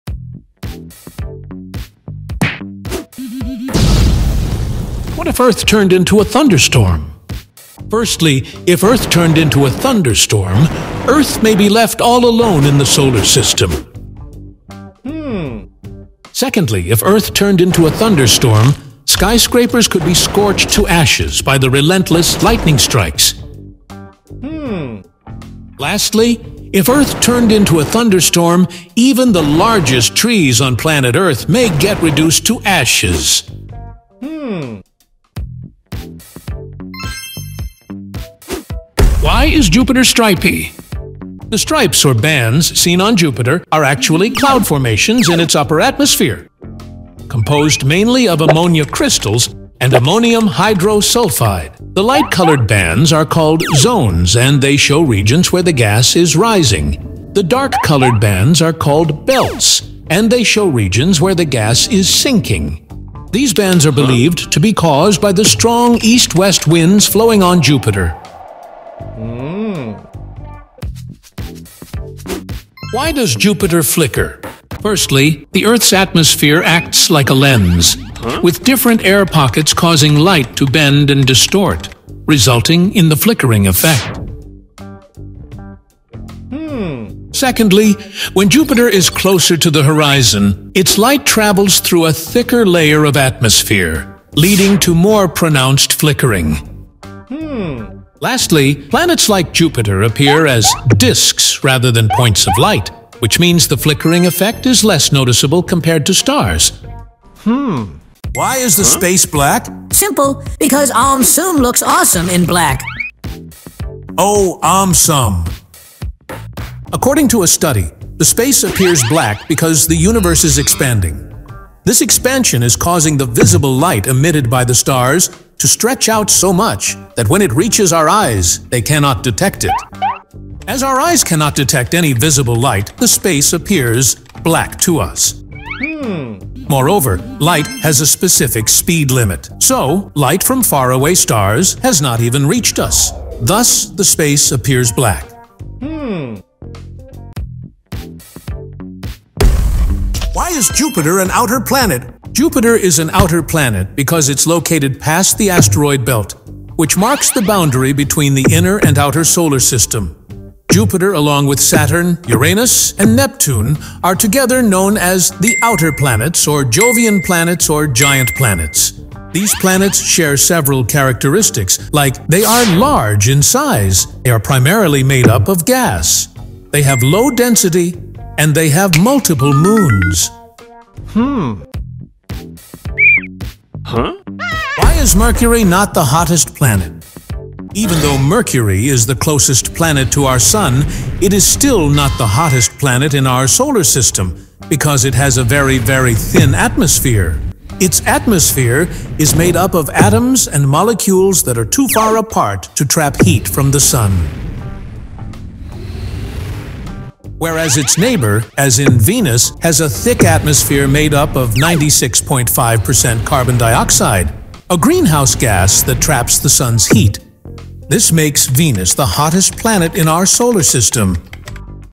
What if Earth turned into a Thunderstorm? + more videos
What if Earth turned into a Thunderstorm?
Firstly, if Earth turned into a Thunderstorm, Earth may be left all alone in the Solar System!
Secondly, if Earth turned into a Thunderstorm, skyscrapers could be scorched to ashes by the relentless lightning strikes!
Lastly, if Earth turned into a Thunderstorm, even the largest trees on planet Earth may get reduced to ashes!